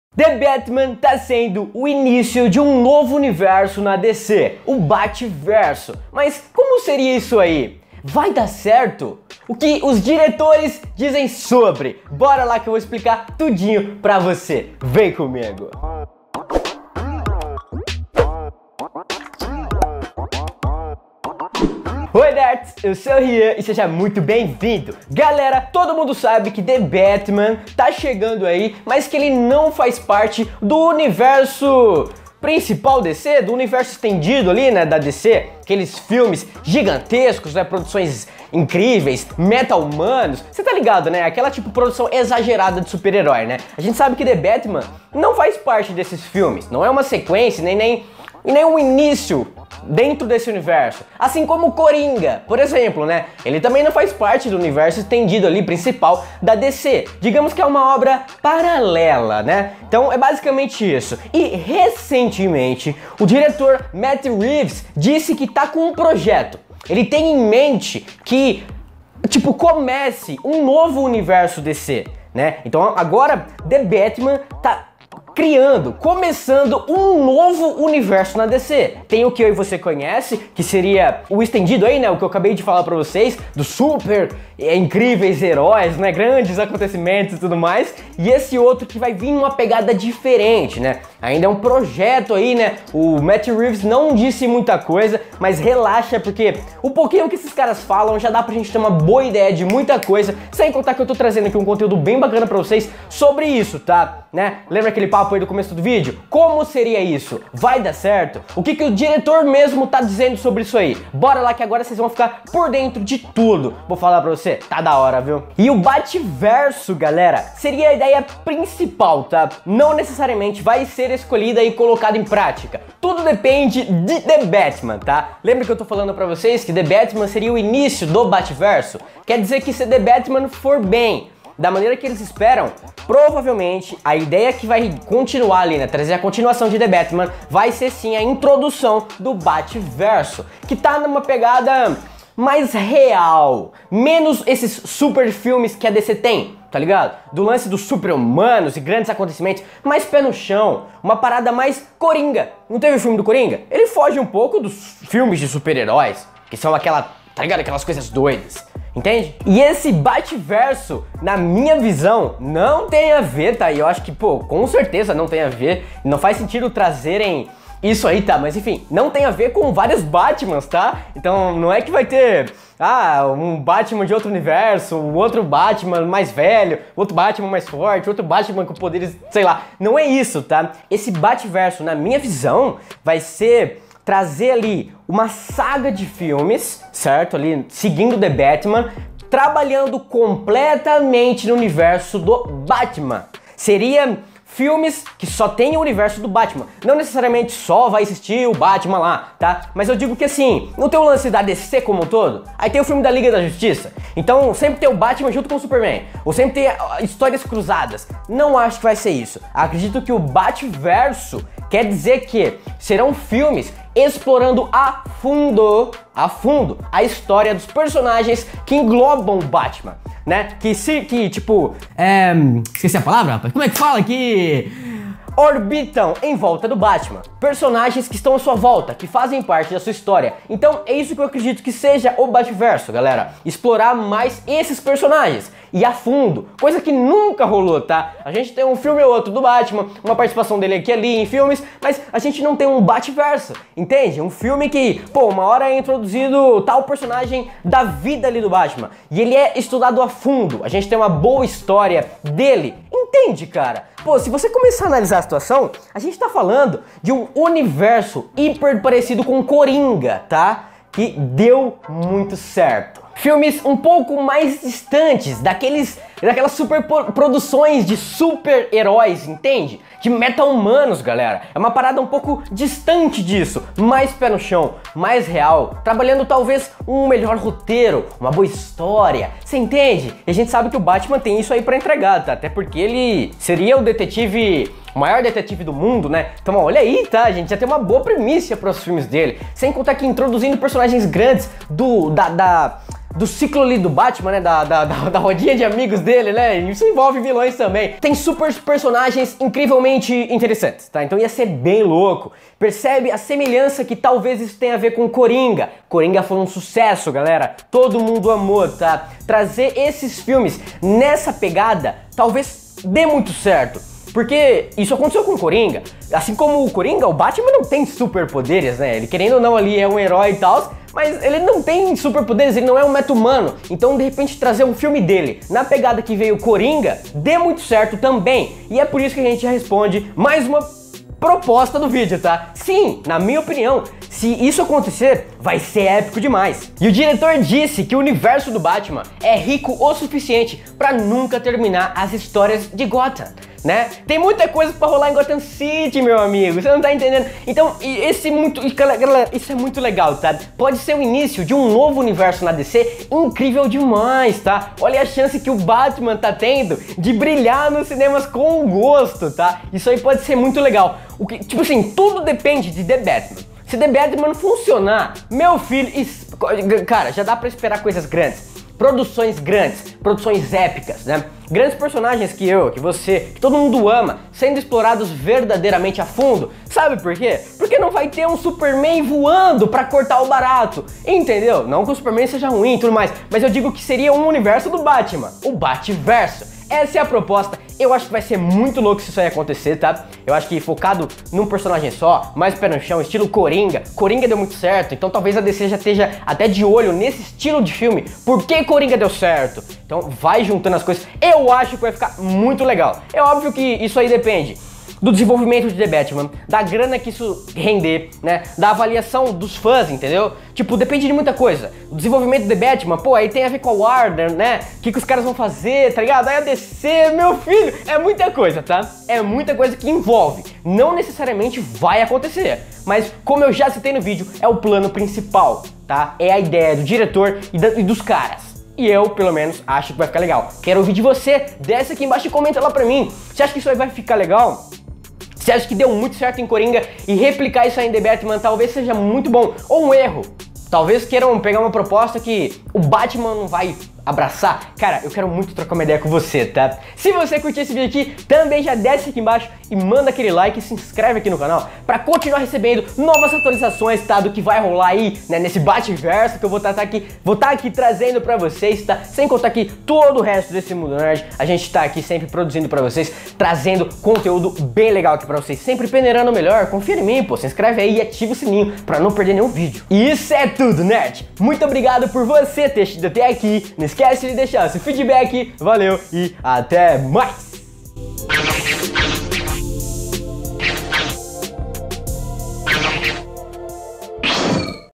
THE BATMAN TEM ROBIN? TEM CORINGA? TEM MUITO MAIS QUE ISSO!
Oe Nerds! Eu sou o Ryan e seja muito bem-vindo!
The Batman tá sendo o início de um novo universo na Dc, o Batverso. Como seria o Batverso?  O Batverso vai dar certo? O que os diretores de The Batman dizem sobre isso? Vou te explicar tudo. Vem comigo!

⚠️ ATENÇÃO! NÃO CLIQUE!

💡 Dá uma conferida!

💡 Fique ligado!

Muito obrigado! Até mais!